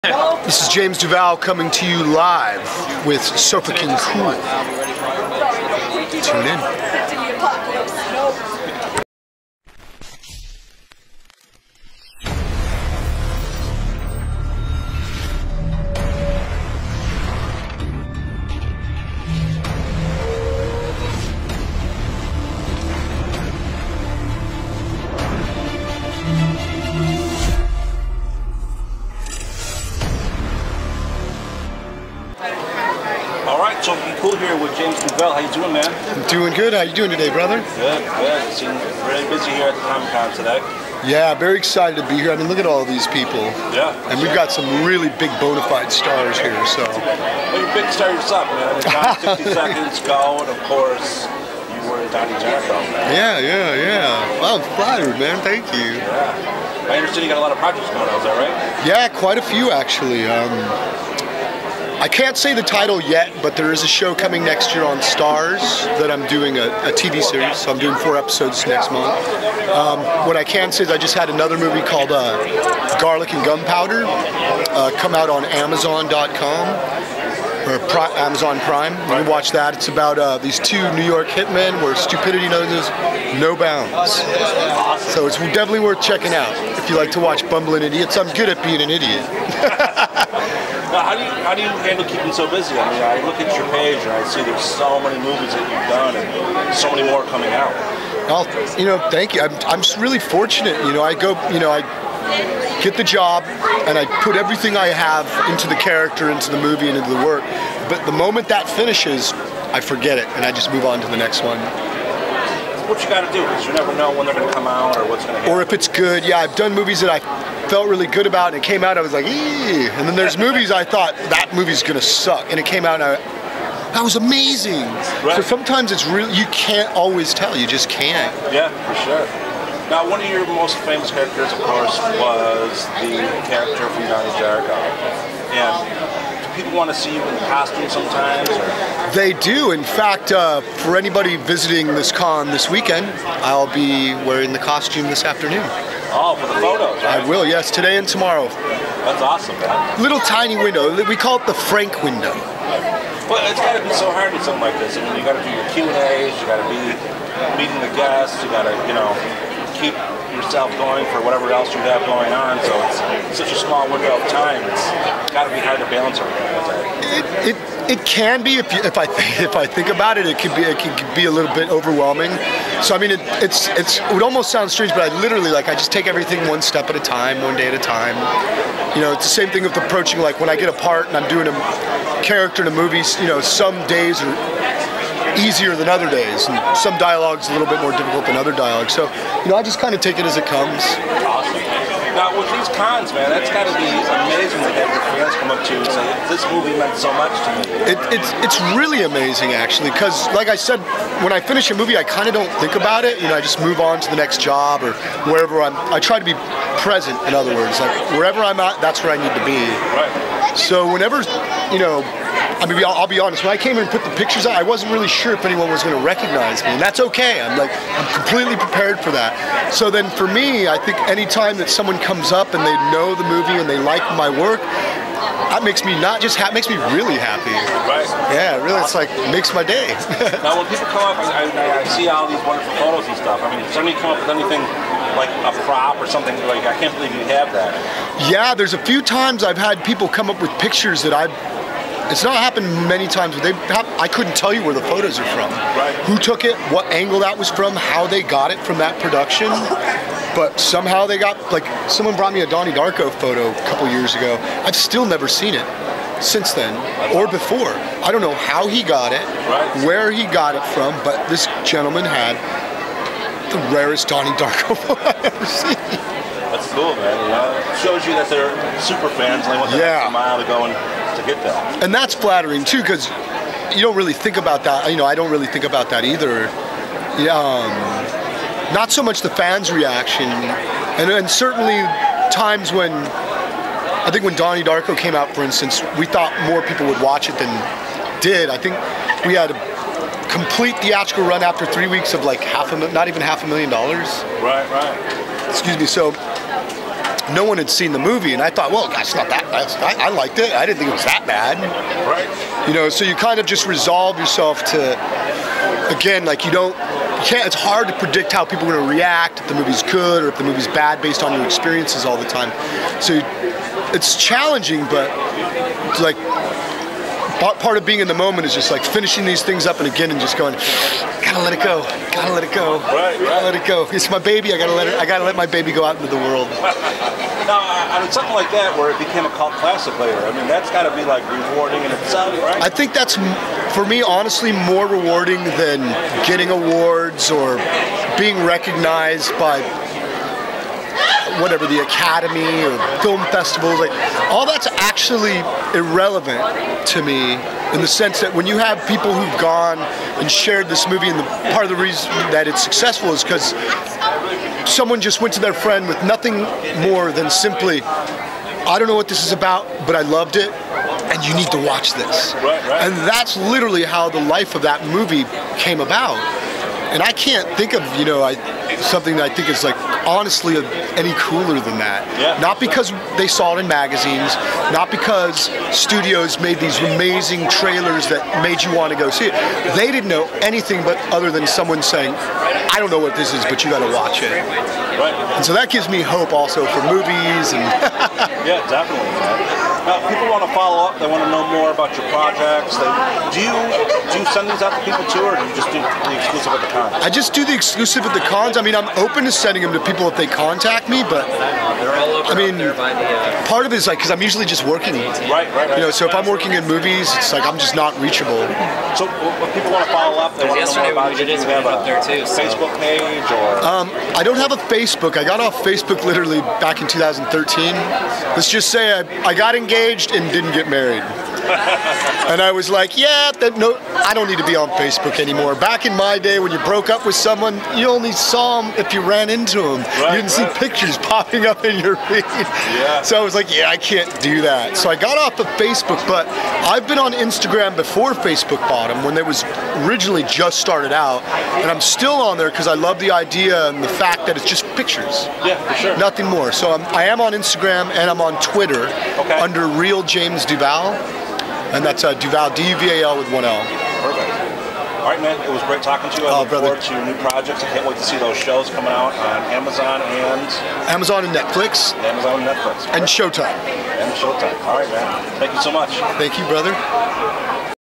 Interview: James Duval Talks about working Cult Film Donnie Darko
Sofa King Cool caught up with James Duval and we talked about his upcoming shows and how Donnie Darko became a cult classic after he thought it was going to bomb